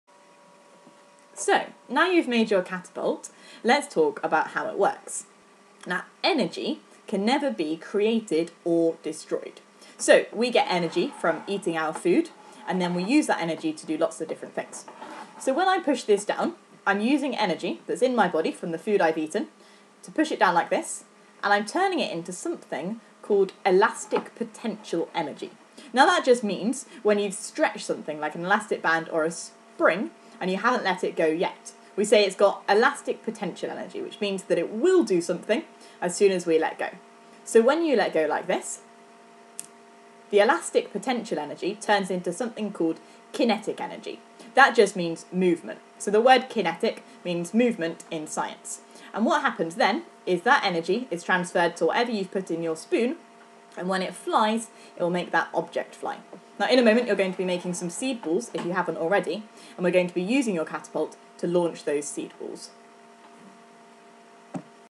How does your catapult work?
Let's break down how a catapult works with this fun explanation on catapult mechanics.